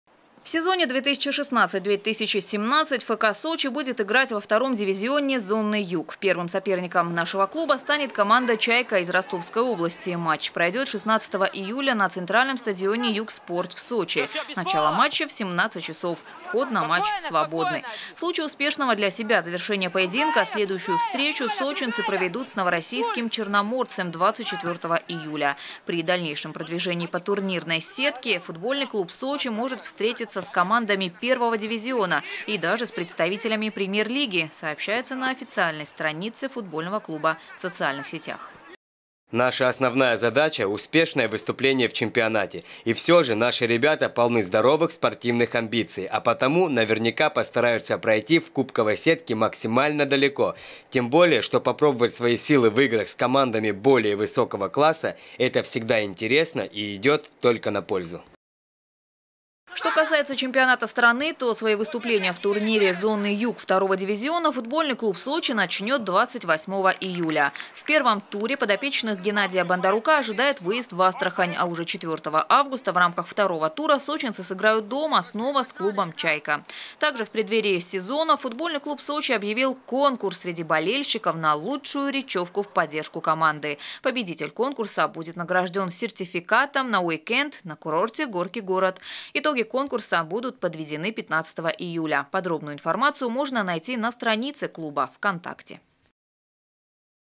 Футбольный клуб «Сочи» открывает сезон. Новости Сочи Эфкате
Сочинские футболисты начинают бороться за Кубок России.
Сочи. Новости.
Телеканал Эфкате.